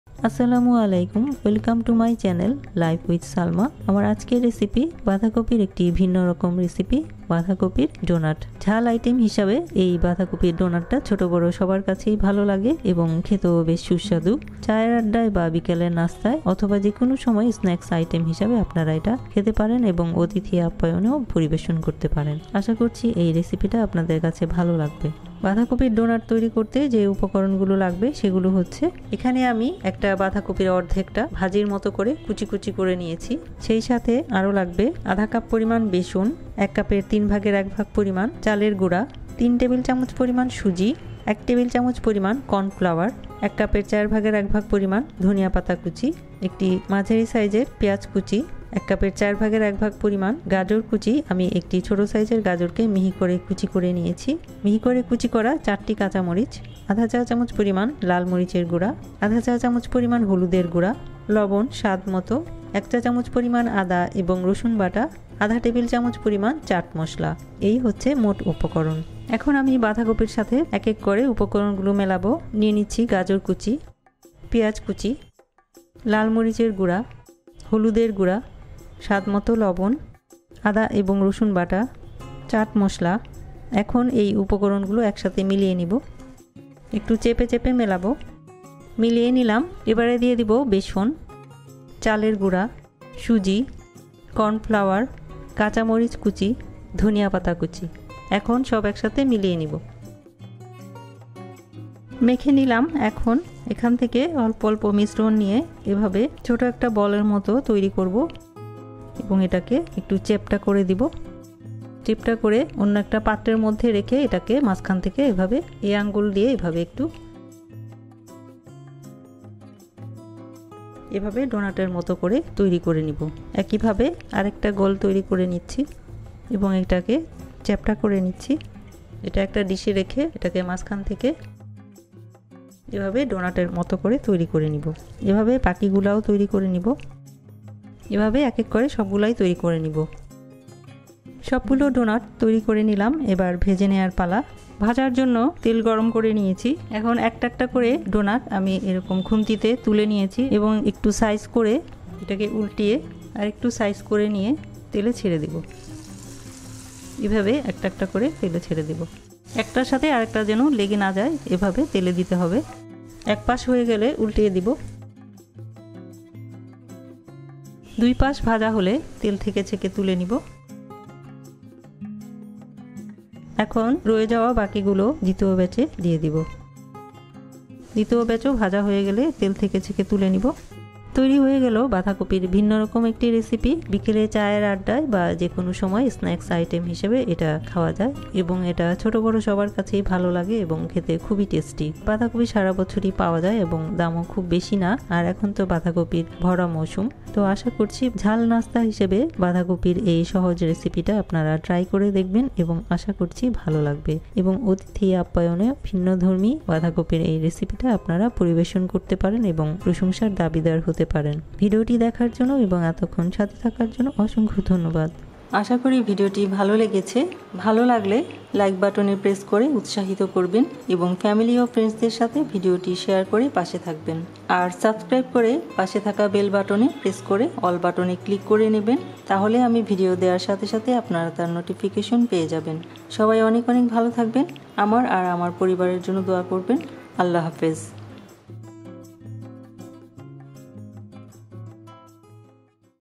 বাধাকপির ঝাল ডোনাট । Badhakopir donut। Cabbage Donuts recipe
আজকে বানাচ্ছি বাধাকপির ঝাল ডোনাট ।  বানানো খুবই সহজ।  বিকেলে চায়ের আড্ডায় বা যে কোন সময় স্ন্যাক্স  হিসেবে বা অতিথি আপ্যায়নে এই ঝাল ডোনাট পরিবেশন করে সবার মন কেড়ে নিতে পারেন।